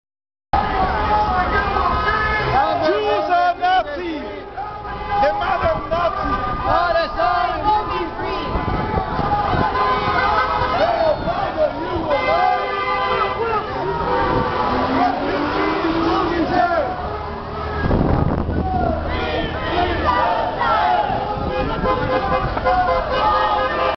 Man dressed as "Jew" with fake nose and locks at anti-Israel demonstration, Los Angeles - 6/4/2010
The first man seen in this clip is an anti-Semite dressed up as a Jew with a plastic "hooked-nose" and a fake sidelocks (or payos). This fellow has become a staple at Left-wing anti-war and anti-Israel demonstrations in Los Angeles. He usually carries an Israel flag with swastikas on it. I have never seen any of the other demonstrators object to his presence.